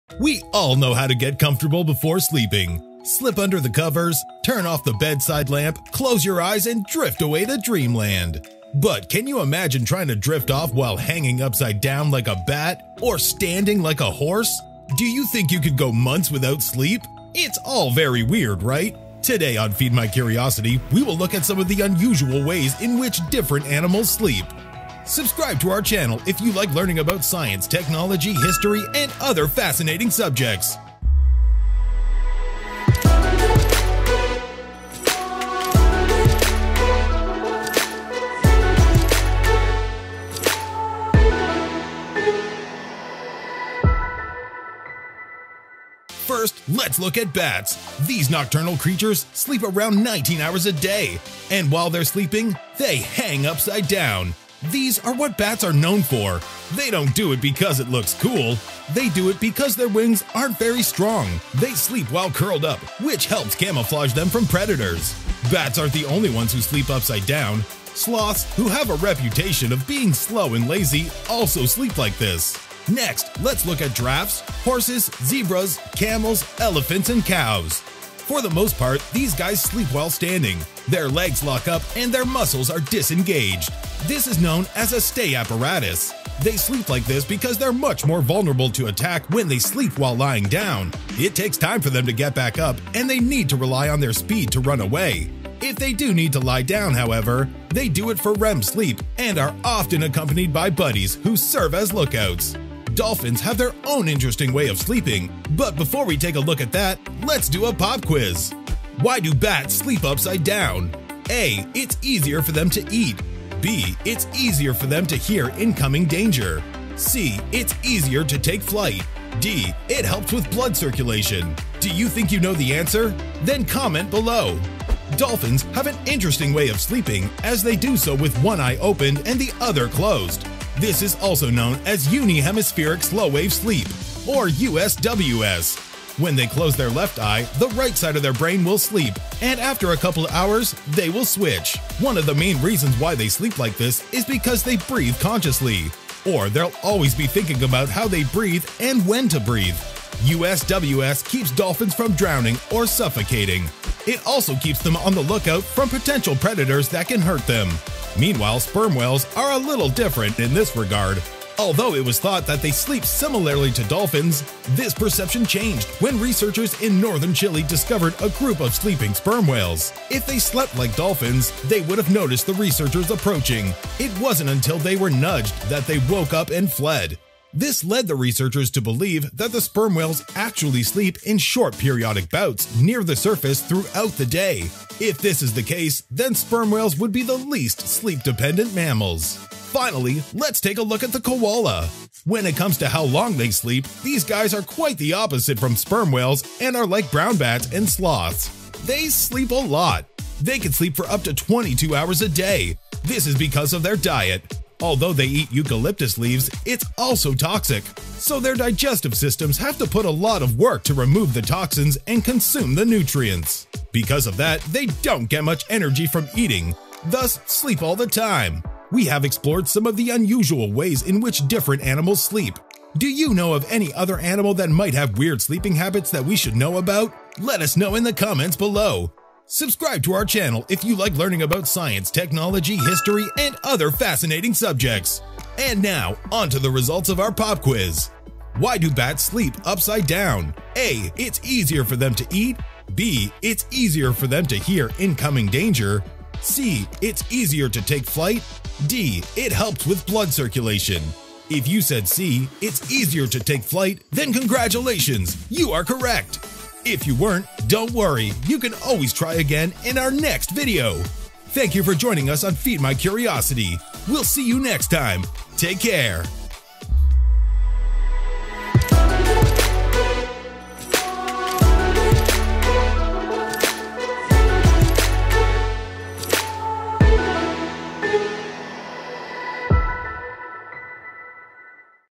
Unusual Sleeping Habits of Different Animals
We all know how to get comfortable before sleeping: Slip under the covers, turn off the bedside lamp, close your eyes, and drift away to dreamland. But can you imagine trying to drift off while hanging upside down like a bat? Or standing like a horse? Do you think you can go months without sleep? It's all very weird, right?

Today on Feed My Curiosity, we will look at some of the unusual ways in which different animals sleep. Let's find out!